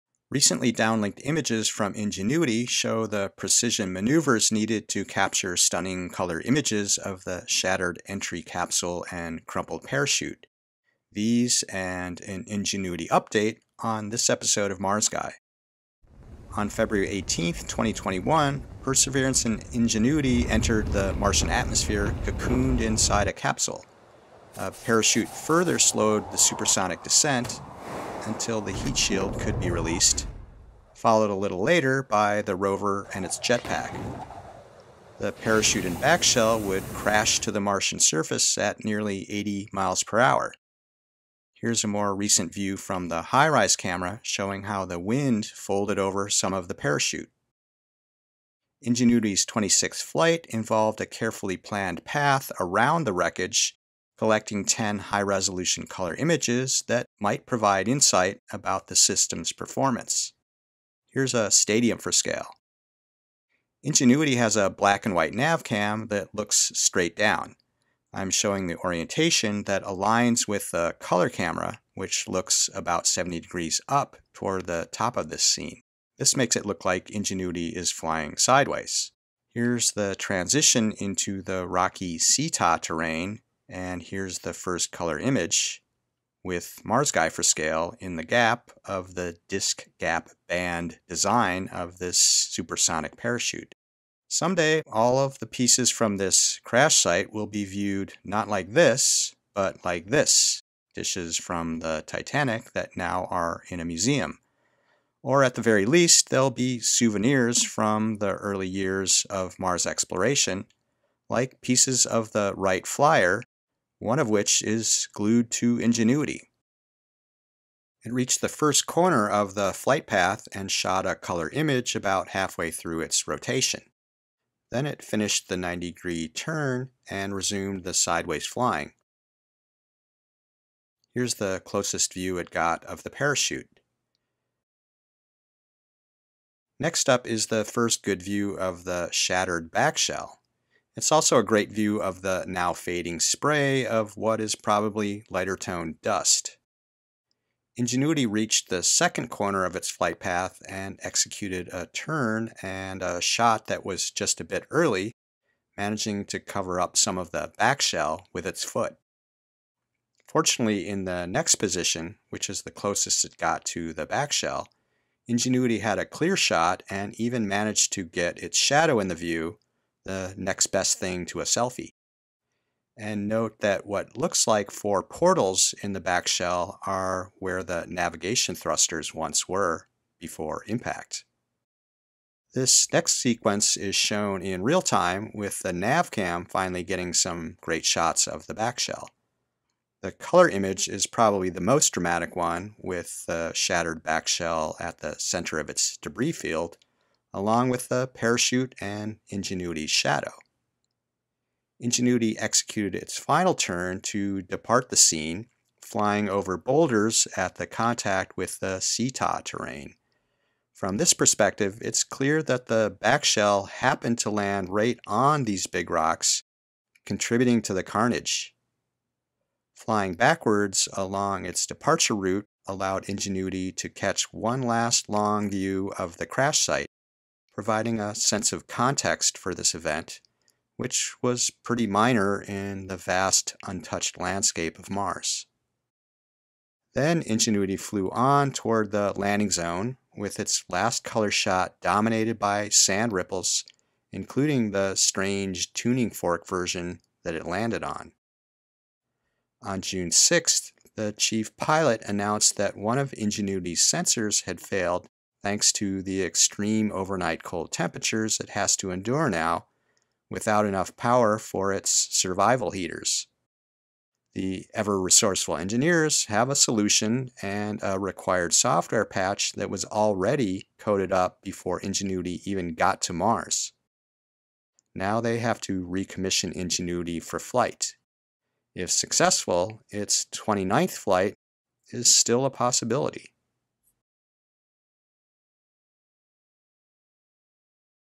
New Ingenuity flight video of wreckage, plus health update
Episode 62
Recently downlinked images from Ingenuity show the precision maneuvers needed to capture the stunning color images of the shattered entry-capsule and crumpled parachute. These images and an Ingenuity update are presented.